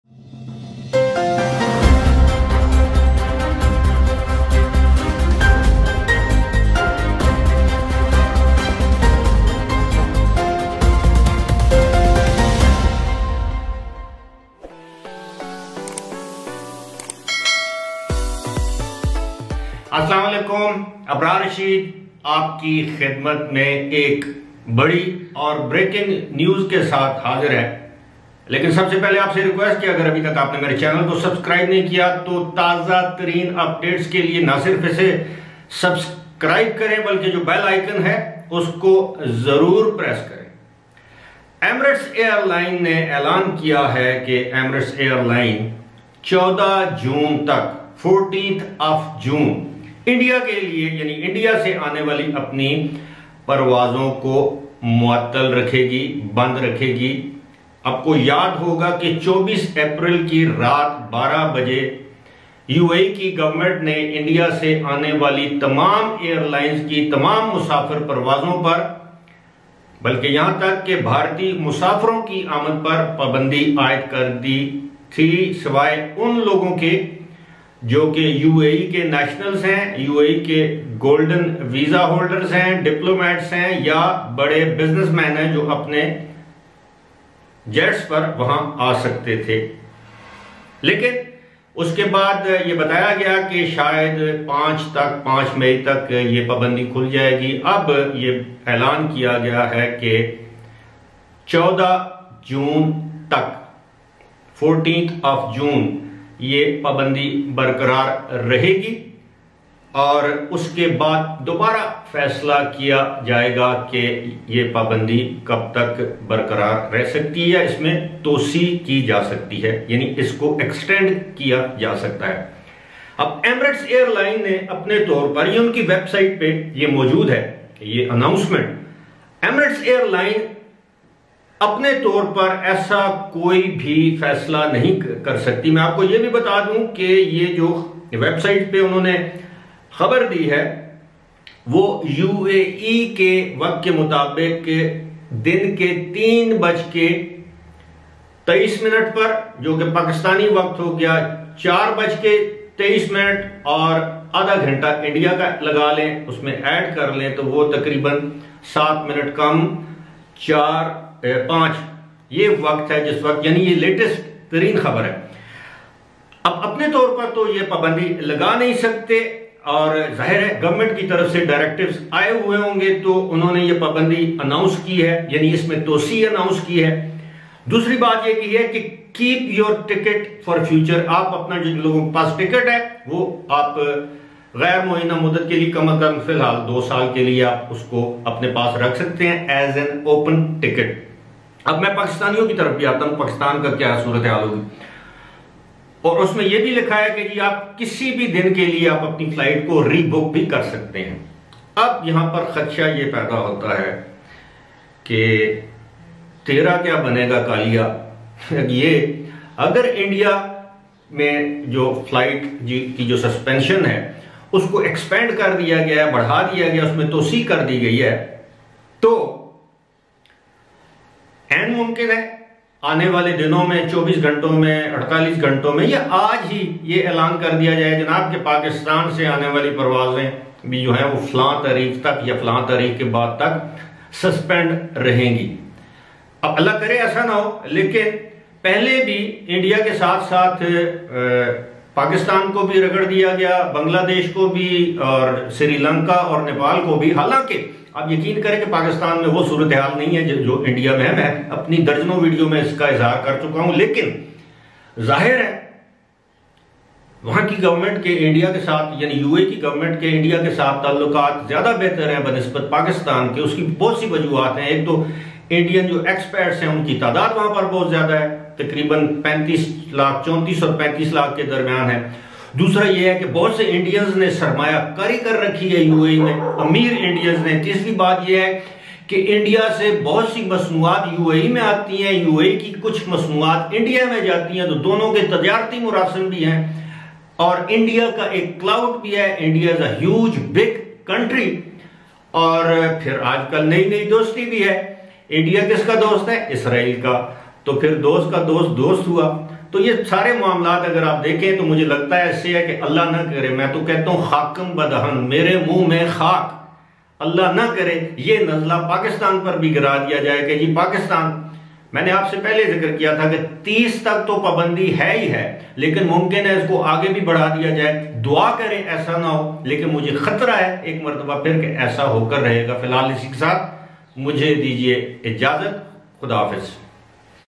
Indian flights ban extended what next for Pakistan
The UAE government has extended passenger flights ban till 14th of June.